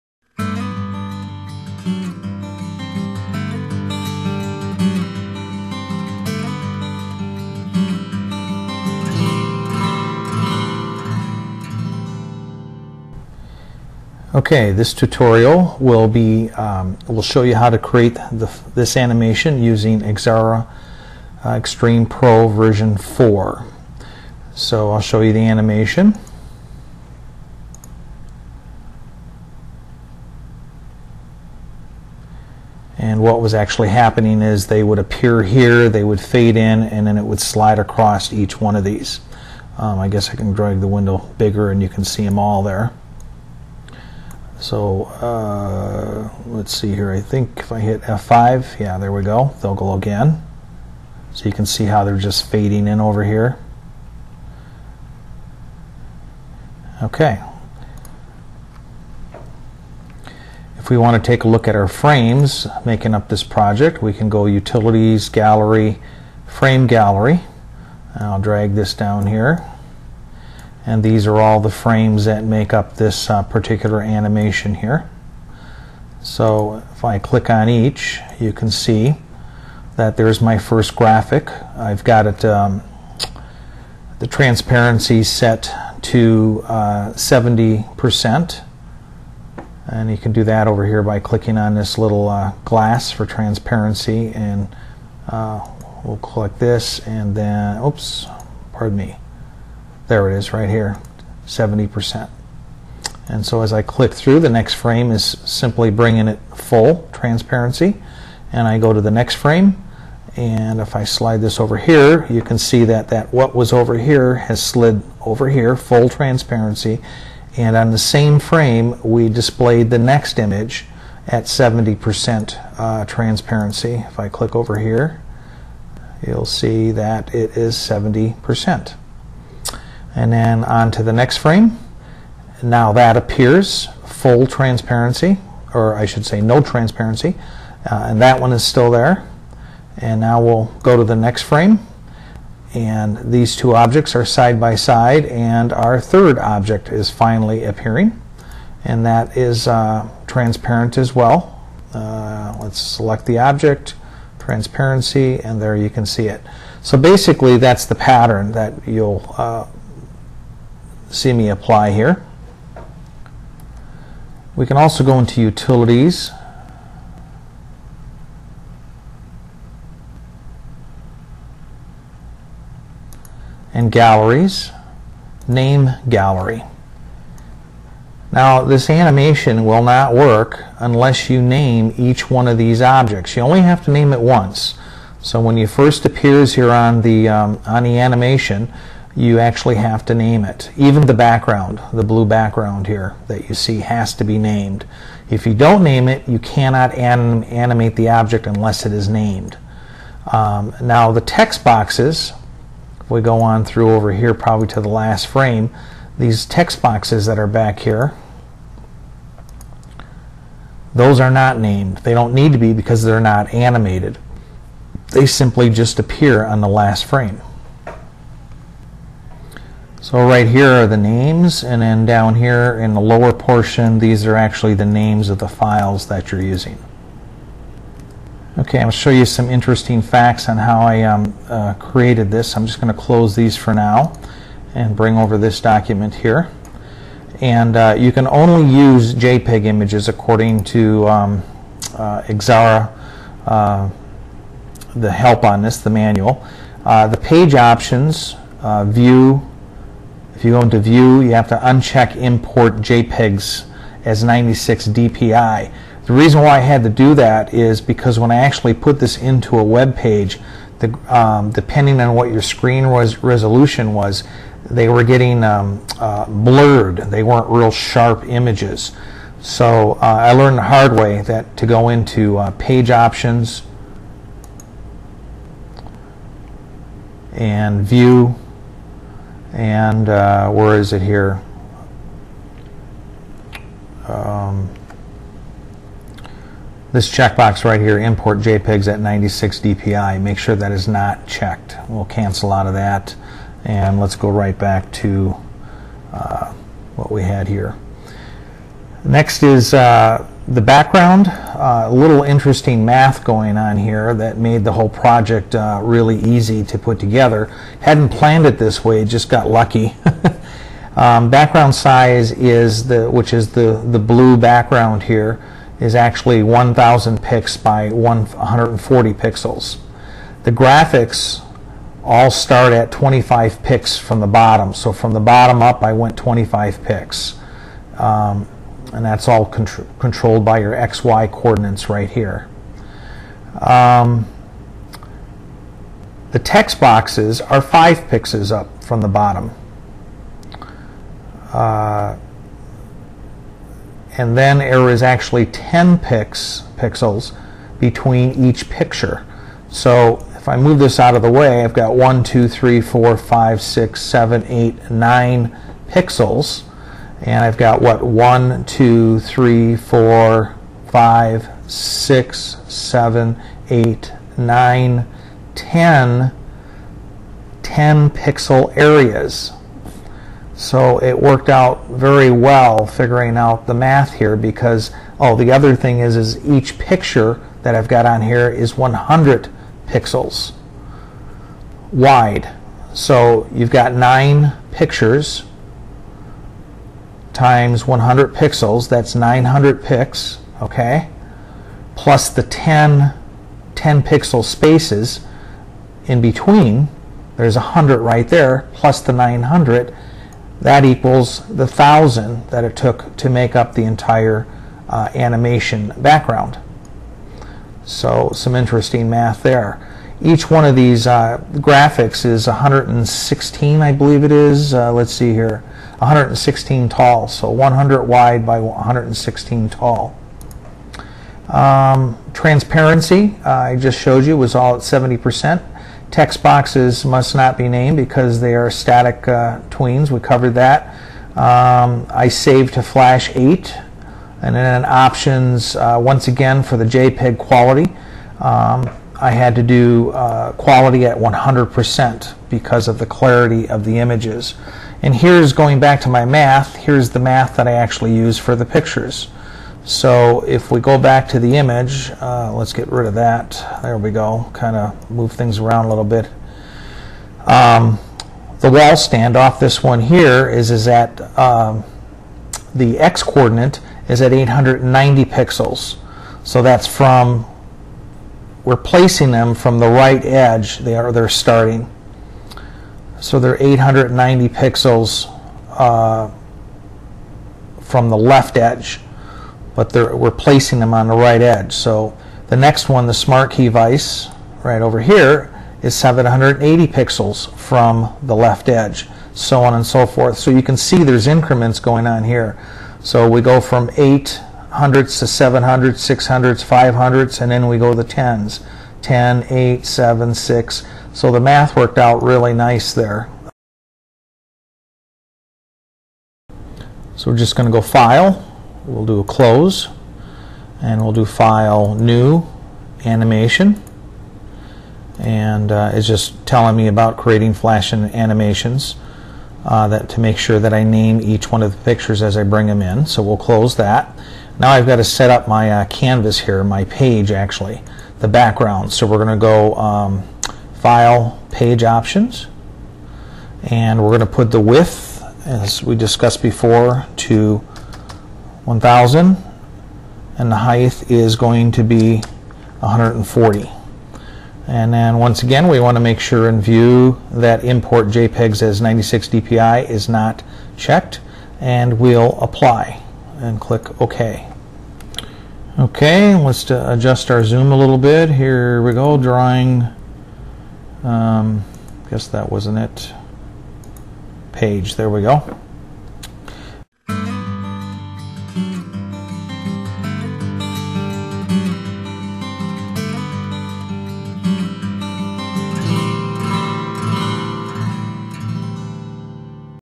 TUT: Xara Sliding Images Flash Animation P1
Creating a flash animation of images sliding in using Xara Xtreme v4. This is part 1 of 3.